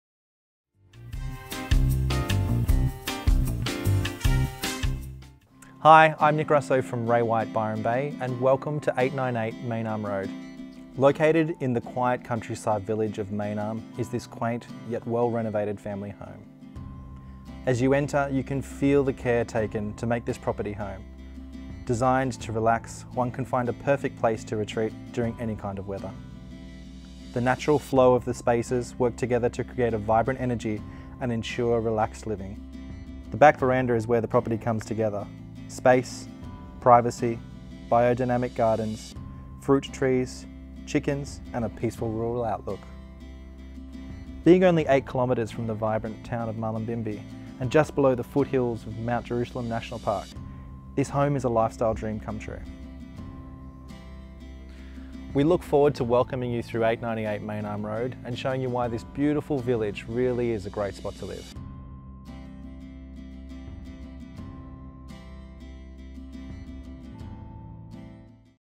Brett Connable Real Estate presents 898 Main Arm Road, Main Arm
Located in the quiet countryside village of Main Arm is this quaint, yet well renovated family home.

As you enter you can feel the care taken to make this property home.  Designed to relax, one can find a perfect place to retreat during any kind of weather…

The natural flow of the spaces work together to create a vibrant energy and ensure  relaxed living.

The back verandah is where this property comes together.
Space…  Privacy…. Biodynamic gardens, fruit trees… Chickens…  and a peaceful rural outlook…

Being only 8 kilometers from vibrant town of Mullumbimby and just below the foothills of the Mount Jerusalem National Park, this home is a lifestyle dream come true.

We look forward to welcoming you through 898 Main Arm Road and showing you why this beautiful village really is a great spot to live.